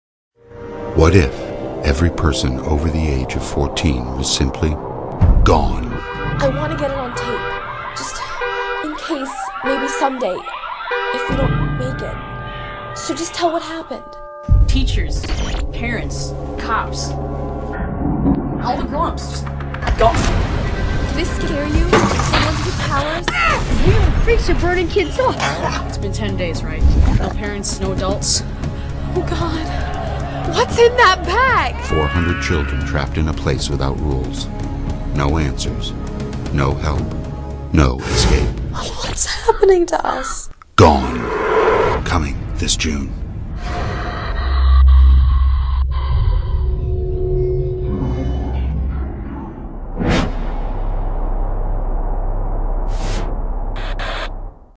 Gone Trailer
Gone by Micheal Grant: go and read it. NAO.

It is the best book I've ever read, right up there with the Golden Compass.

I saw this trailer, and was like OMG, so I made this out of it. It sucks, but heck, I don't really care. I love this. GO READ GONE NAO!!! Actually, buy it, because it is something you'll read over and over again